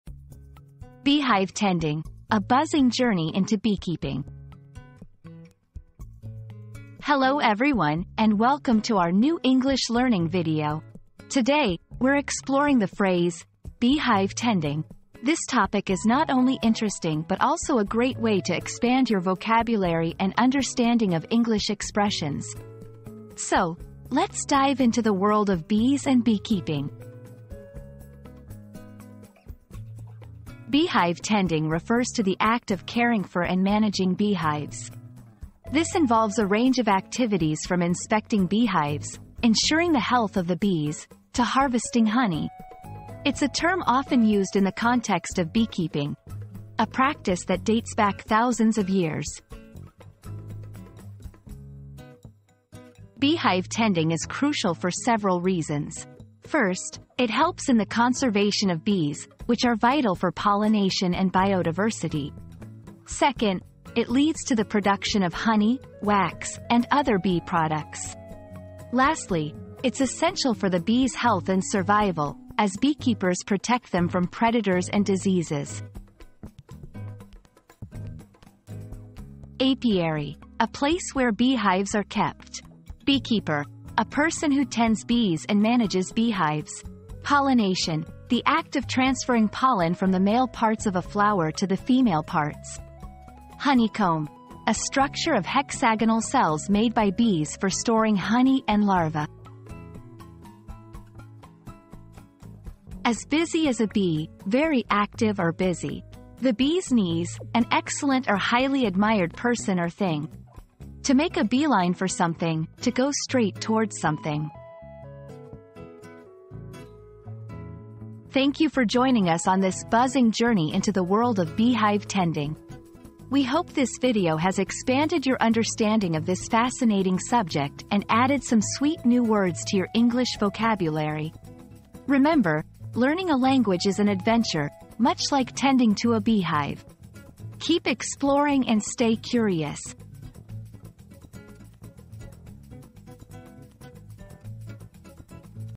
Beehive Tending: A Buzzing Journey into Beekeeping
00:00 • Introduction - Beehive Tending: A Buzzing Journey into Beekeeping
00:30 • Understanding 'Beehive Tending'
00:52 • Why is Beehive Tending Important?
01:19 • Vocabulary in Beekeeping
01:42 • Phrases Related to Beehive Tending